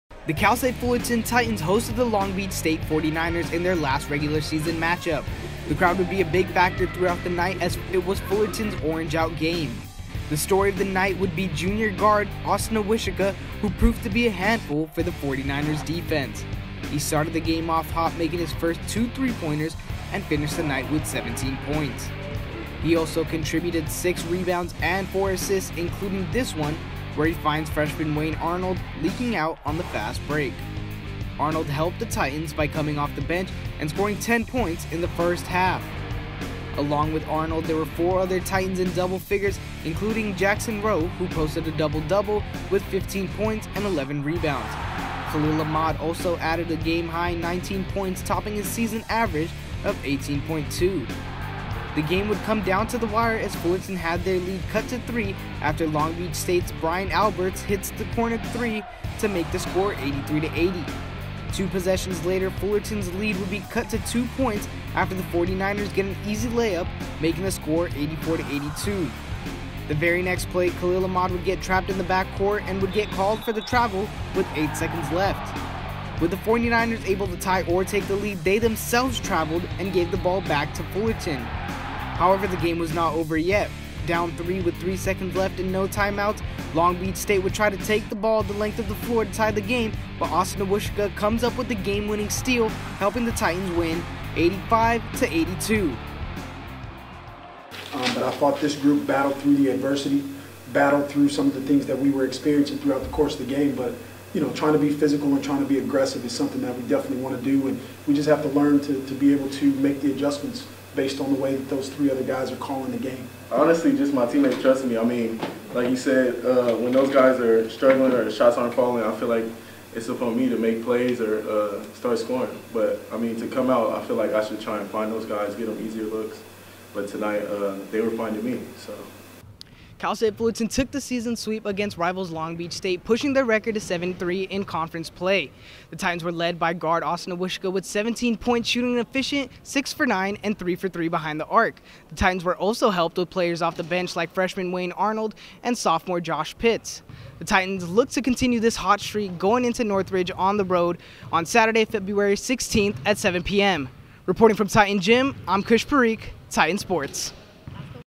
CSUF Men's Basketball vs Long Beach State 2/13/19
Fullerton's Mens Basketball hosted Long Beach State in their annual Orange Out game. The game came down to the wire but the Titans edged out the 49ers 85-82.

Austen Awoskia: 17 pts, 4 steals (6-9 FG) (3-3 3PT)
Khalil Ahmad: 19 pts, 5 assists (6-14 FG)
Jackson Rowe: 15 pts, 11 rebs (5-9 FG)